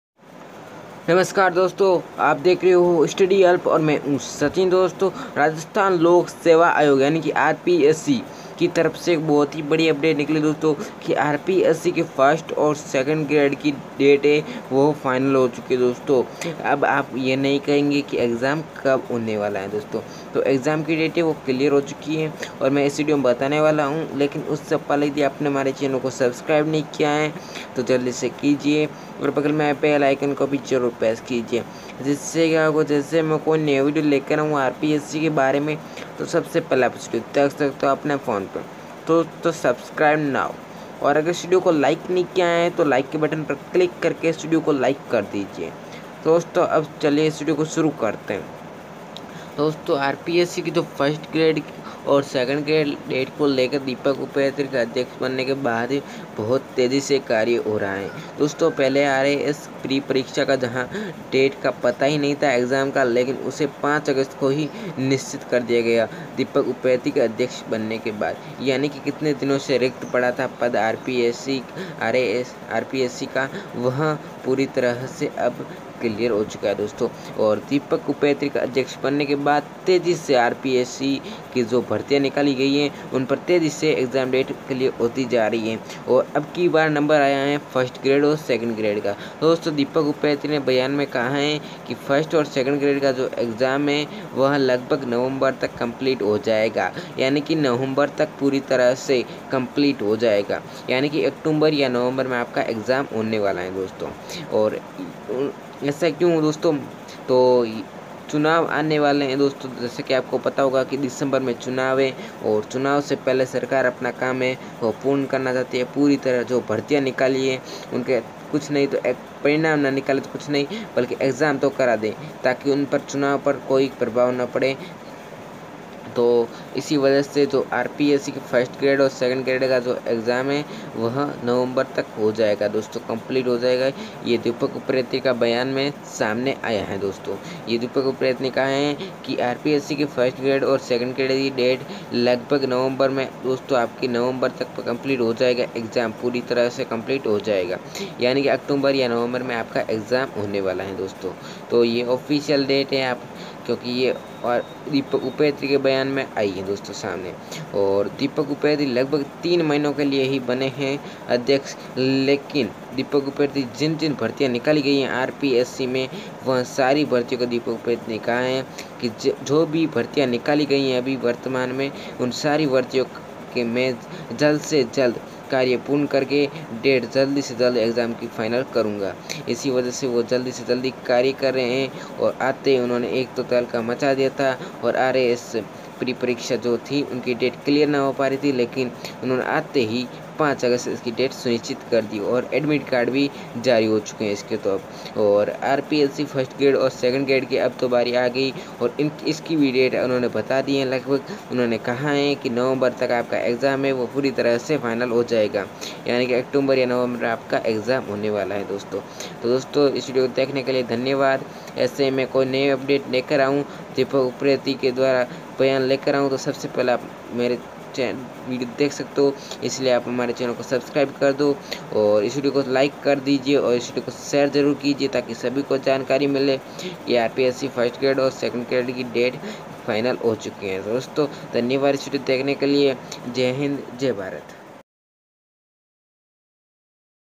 RPSC 1st Grade & 2nd Grade Exam Date 2018//Deepak Upreti ka Bayan Latest Update
RPSC 2018
दीपक उप्रेती का बयान नवम्बर तक कम्पलीट हो जाएगी 1st and 2nd ग्रेड की परीक्षाएं Latest News